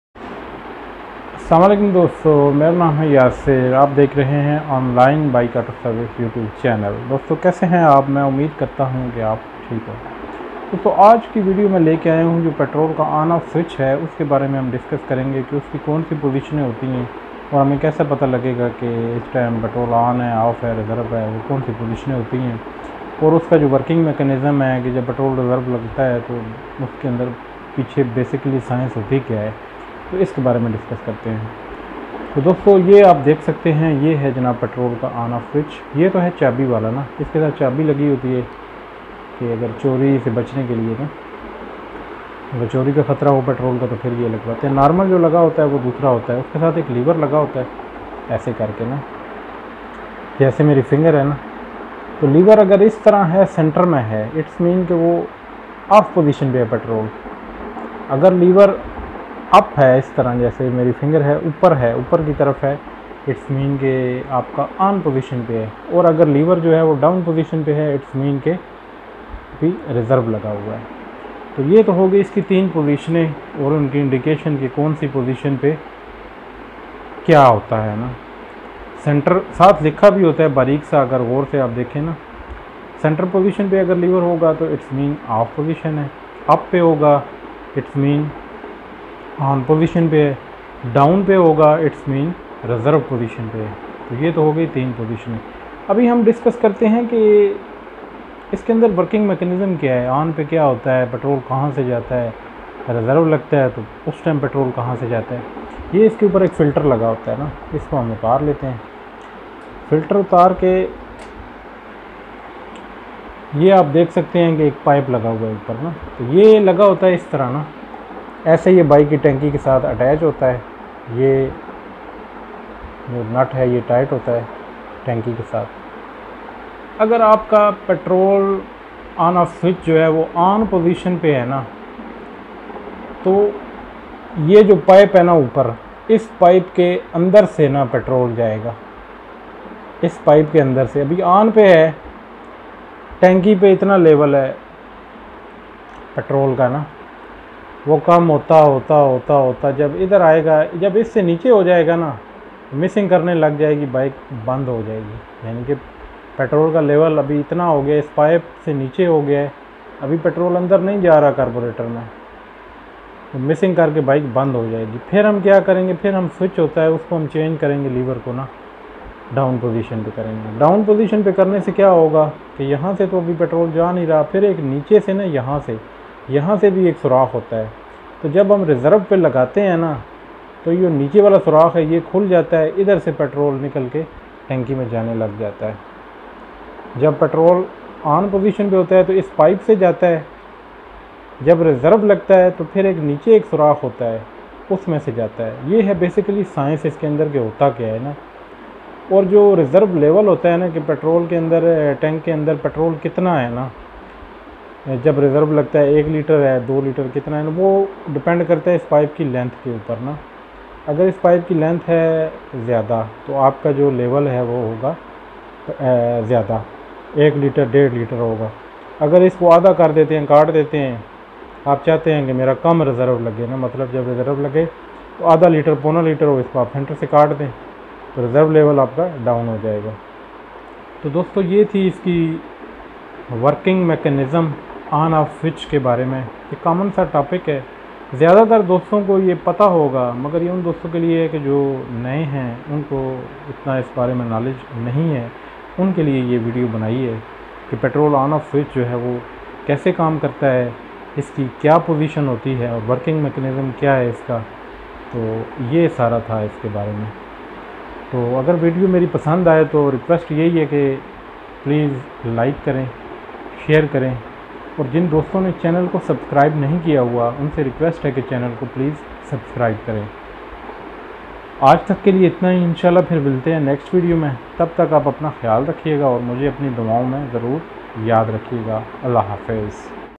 petrol switch in bike | petrol on off in bike | petrol reserve in bike
today this video is about petrol switch in bike , petrol on off in bike , petrol reserve in bike. what is working mechanism of it and which are 3 postion of it and what meant for these positions.